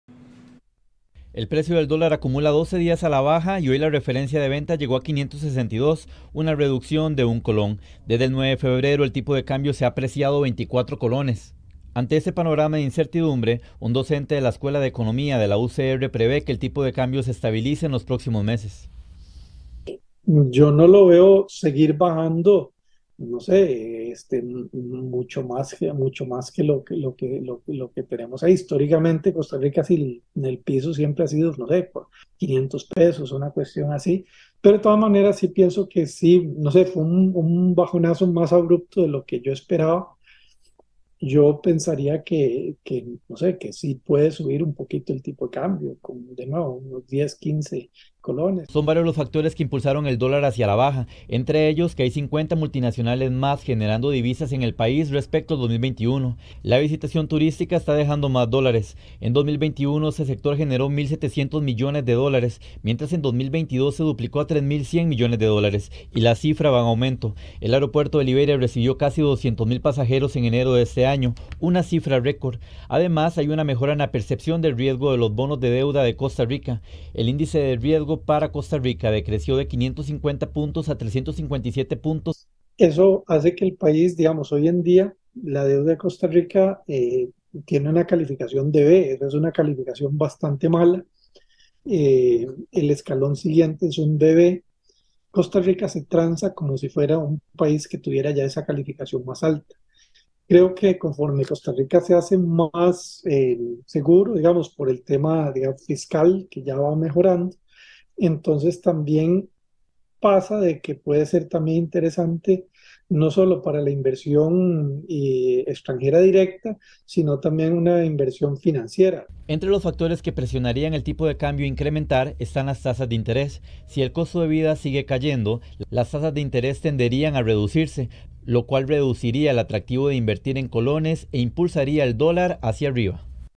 Experto asegura que dólar se mantendrá estable en los próximos meses
Según el experto, la presencia de multinacionales, el turismo y la reforma fiscal aumentan la oferta de dólares.
El índice de riesgo del país decreció de 550 puntos a 357 puntos, mejorando las oportunidades de inversión.